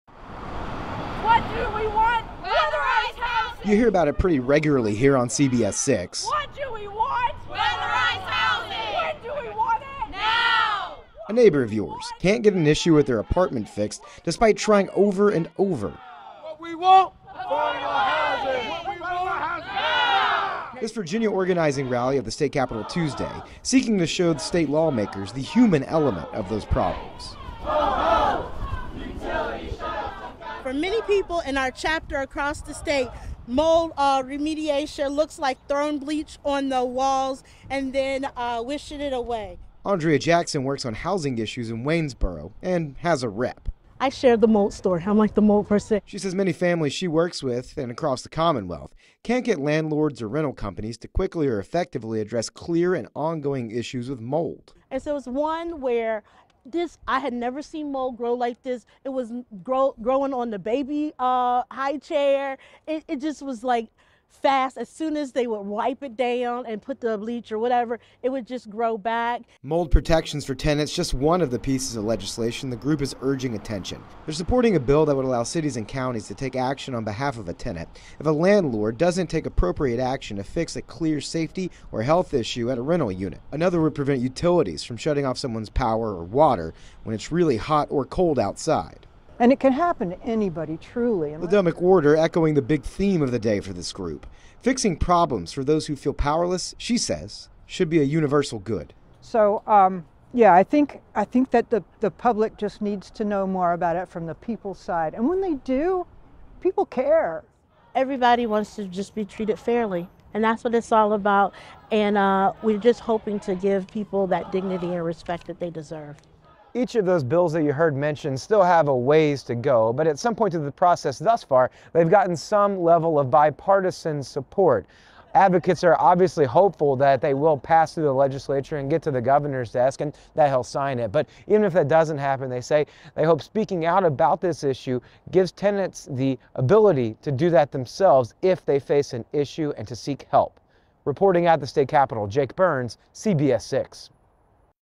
These Virginia renters and advocates are rallying for better housing conditions
A rally hosted by Virginia Organizing sought to show the human element behind housing conditions that go unaddressed.